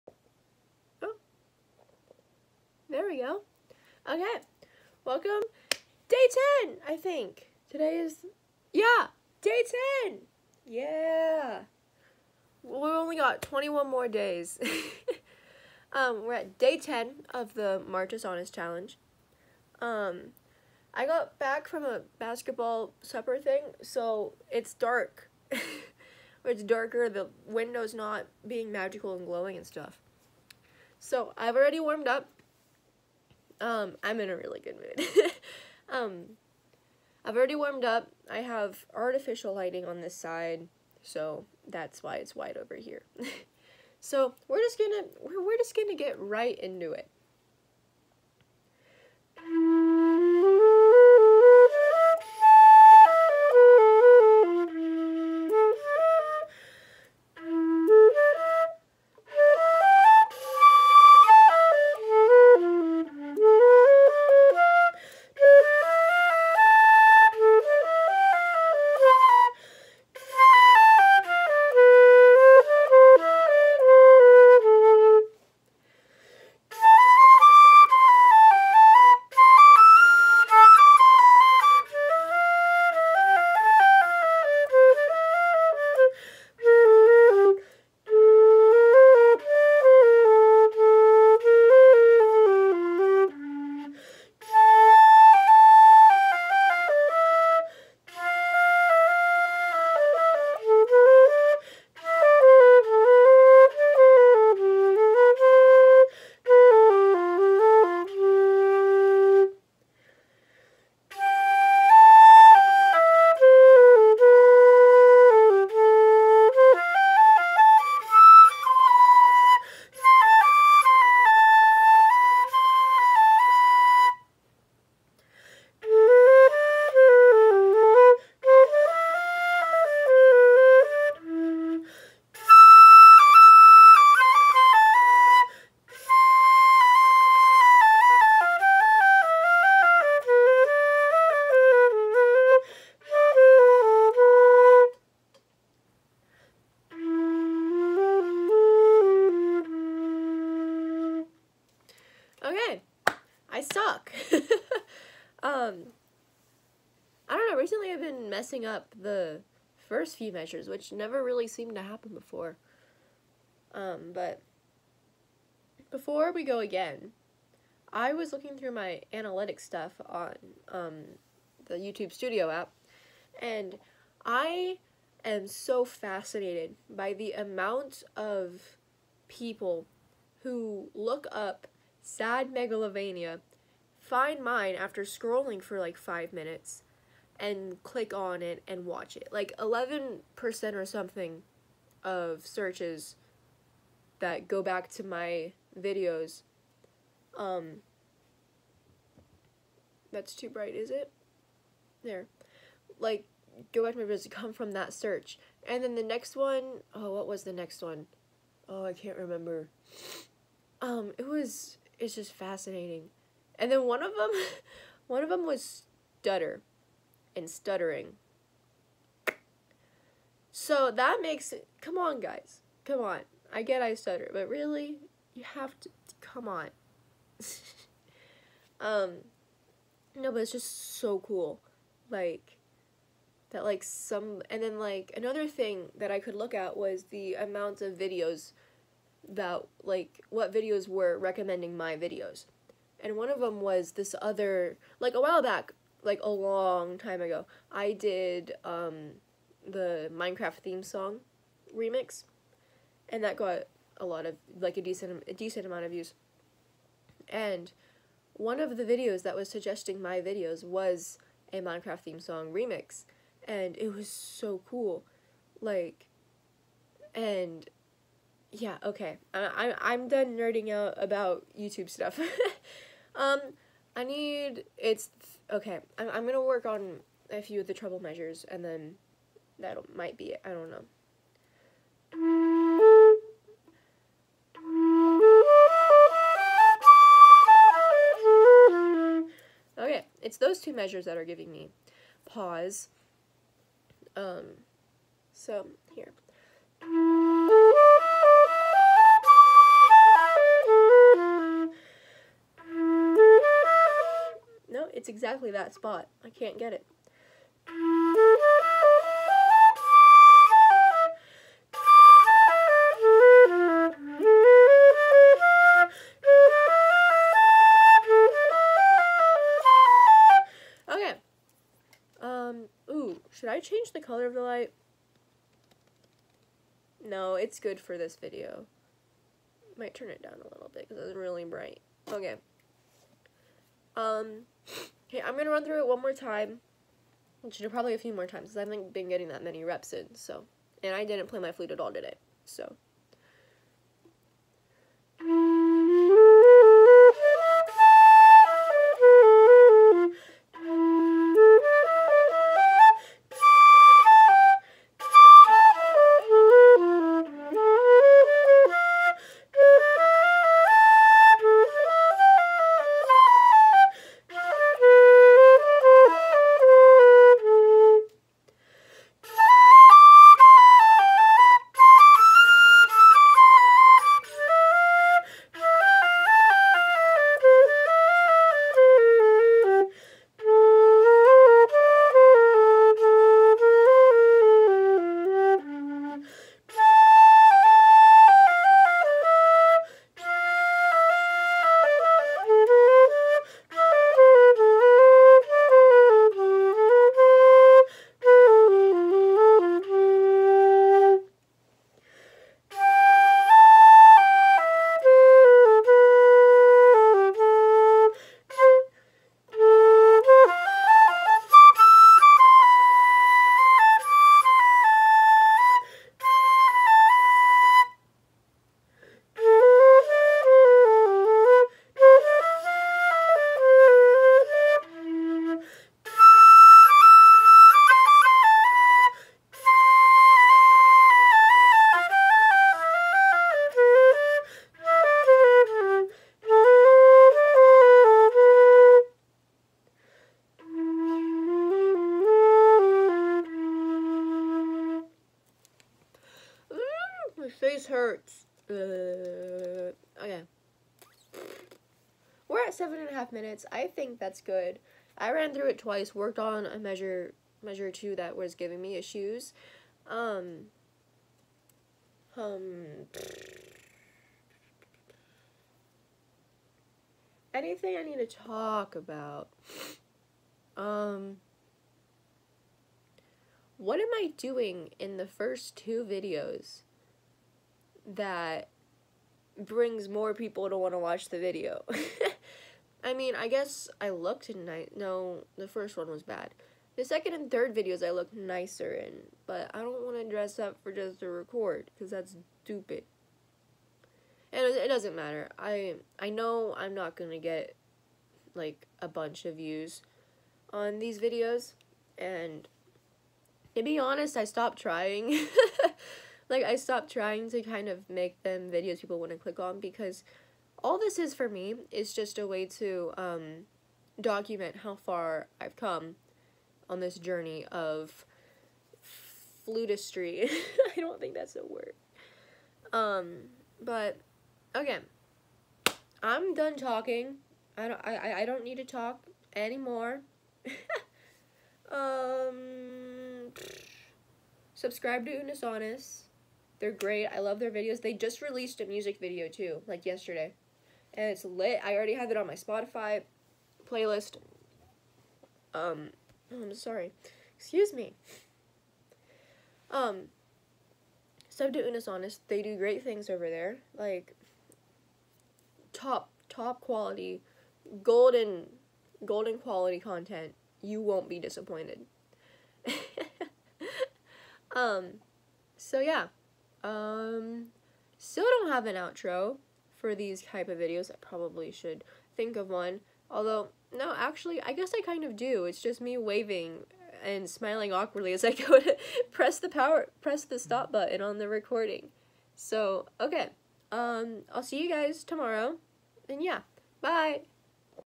50% me nerding out and 50% me playing — MarchusAnnus challenge day 10
We made it all the way to ten days yeahhhhhh

I’m definitely getting worse but hey it’s fine I’m sure everything will turn out okay in the end right?

...riight?